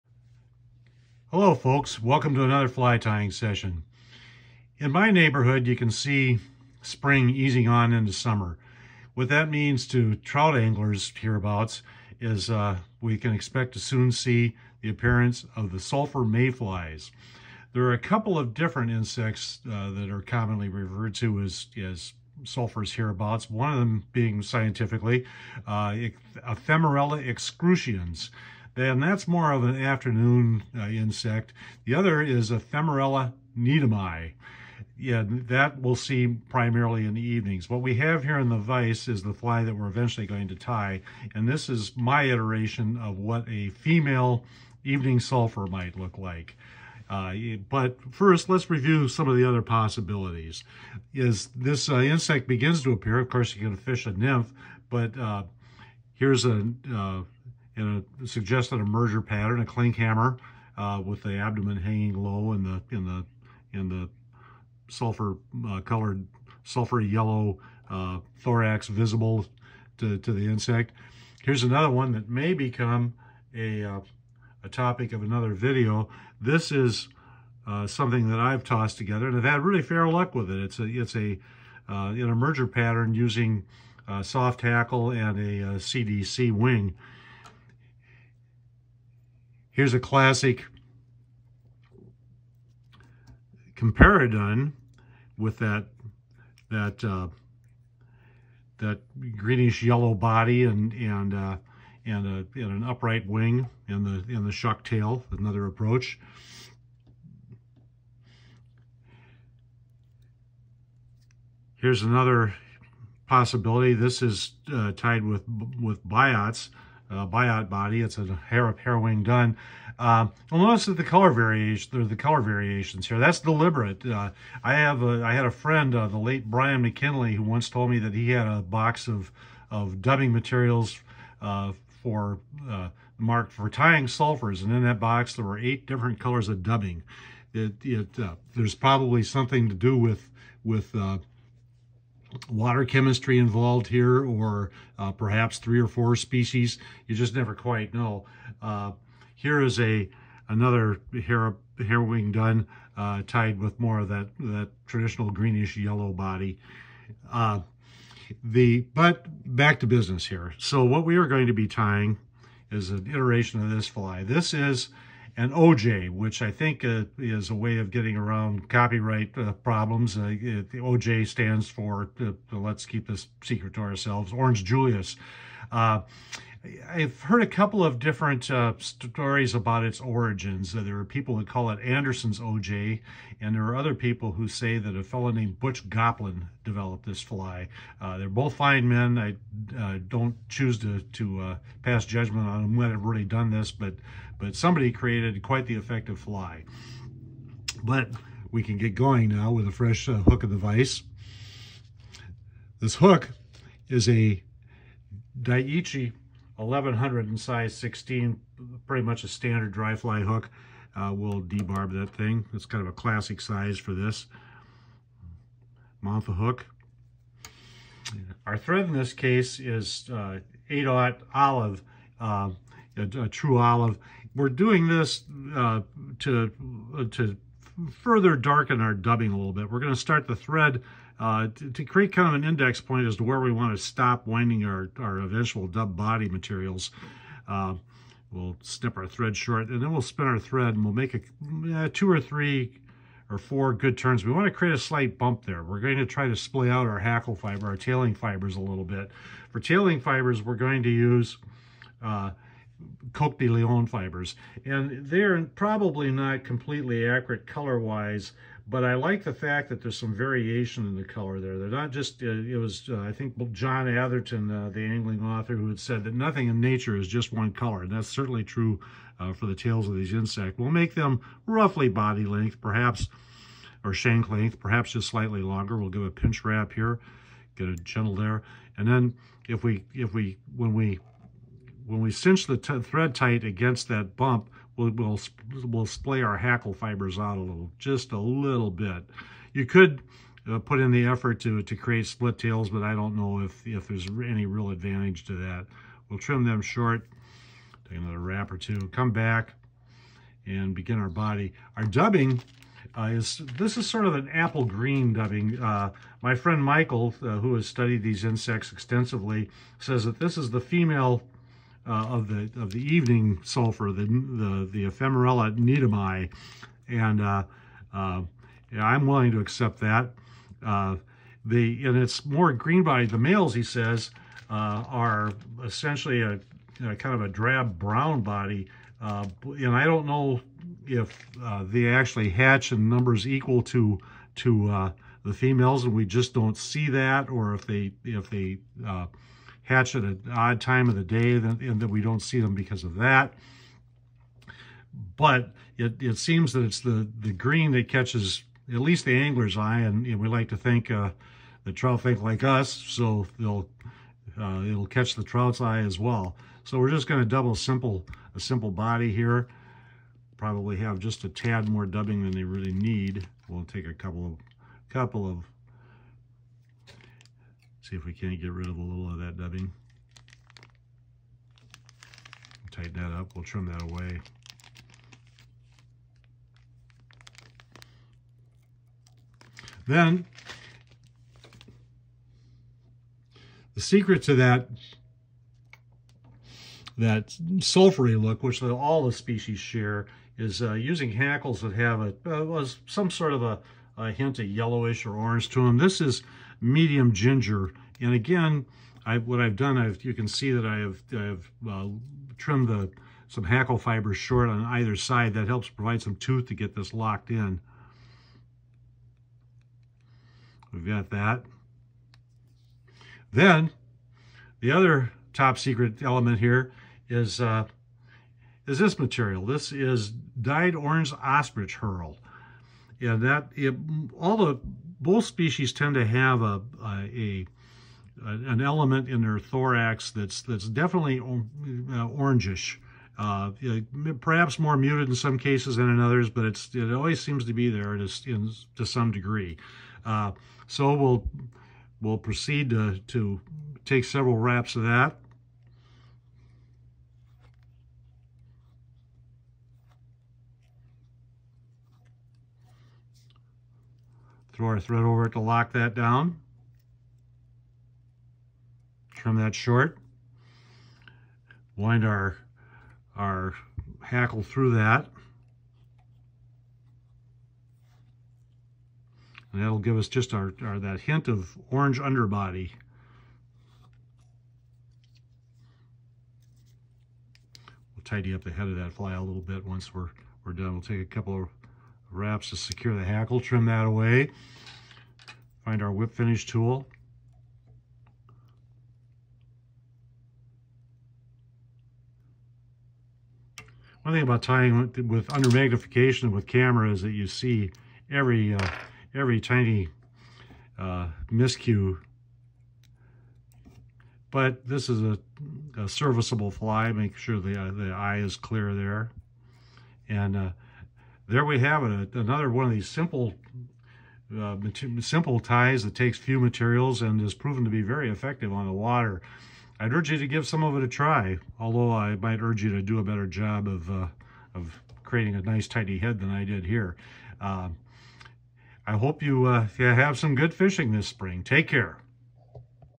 The OJ Sulfur Mayfly
Here's an option for the sulfur hatches we can expect to see in late May well into June.
Hook:  Daiichi 1100, size 16 or other standard dry fly hook in that size.
Thread:  8/O Olive Uni-Thread
Tail:  CDL fibers
Abdomen:  Ultra Fine Caddis Green dubbing
Thorax:  Ostrich herl dyed orange
Hackle:  Medium or light ginger
This recipe is intended as a representation of the female evening sulfur, Ephemerella needhami.  Colors may be varied to match local hatch conditions.